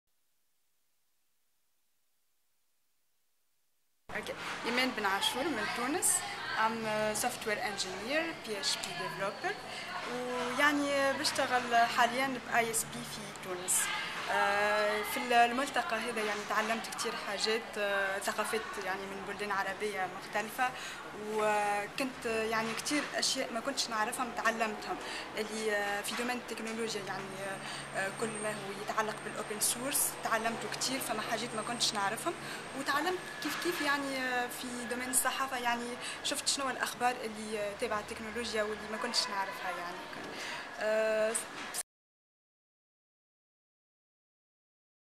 Women Techies 9
Arab Techies, a collective that started with the goal to bring together a varied group of techies who vigorously utilize their IT skills to support their communities on the route of development and social change,and to learn from each other and collaborate on solving common problems in collaboration with Social Media Exchange that provides training and consulting to civil society and nonprofit organizations in Lebanon organized a four-day workshop that hosted 30 female participants. During the workshop, the participants showed off their technical skills through skill sharing sessions, where each participant offered to teach others about a specific tool or approach in applying technology.